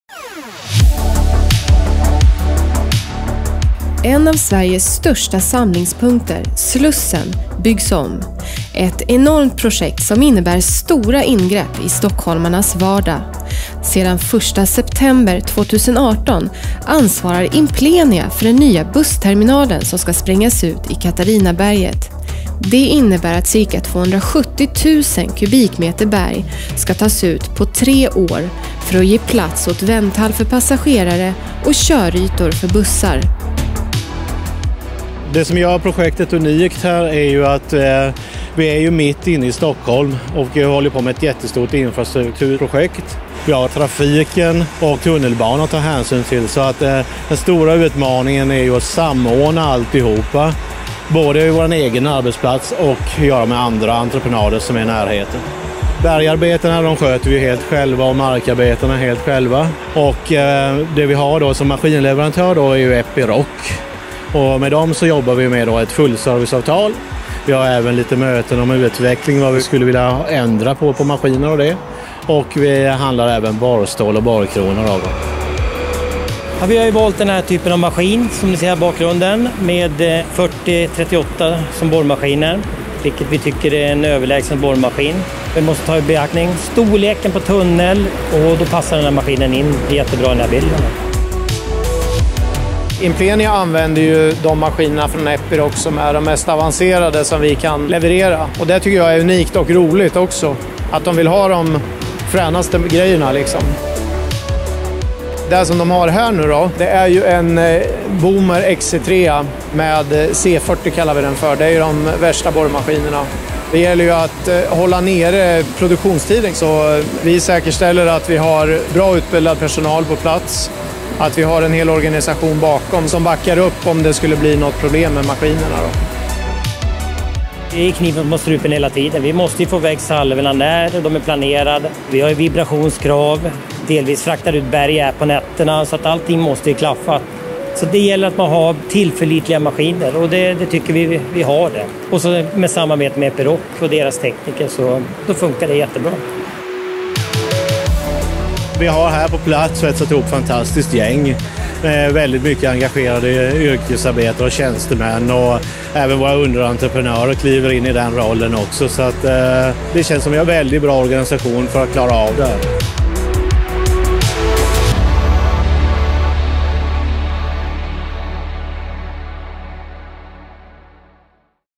Boomer XE3C featuring the powerful COP 4038 rock drill
One of Sweden's largest transit meeting points, Slussen, is being rebuilt. it is a project with many challenges, and it was decided to use the Epiroc Boomer XE3C.